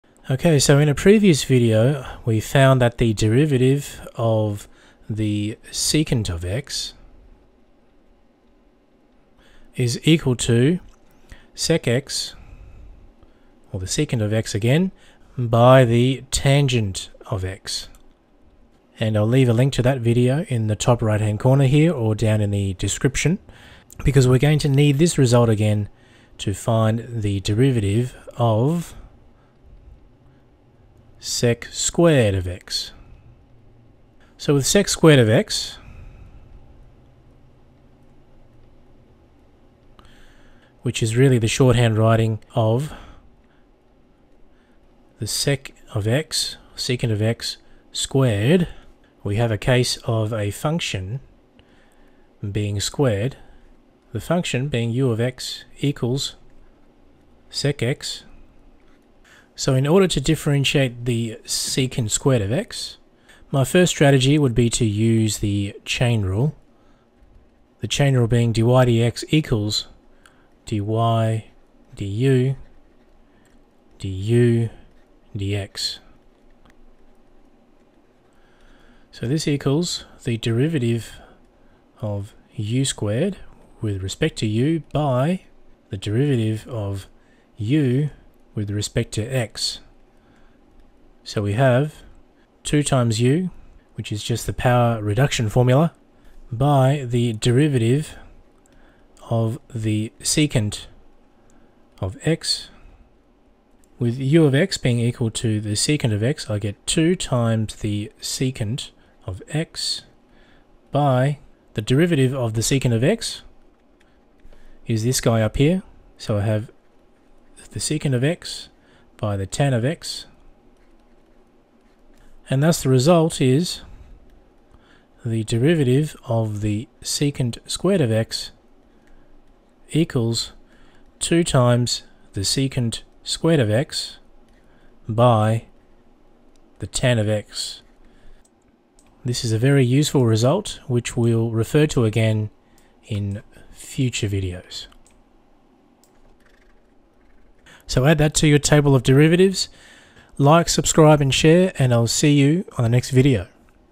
What is the derivative of sec^2(x)? - d/dx[sec^2(x)]
In this video, we use the result d/dx[sec(x)] = sec(x)tan(x) to find the derivative of d/dx[sec^2(x)].

I would really appreciate any small donation which will help me to help more math students of the world.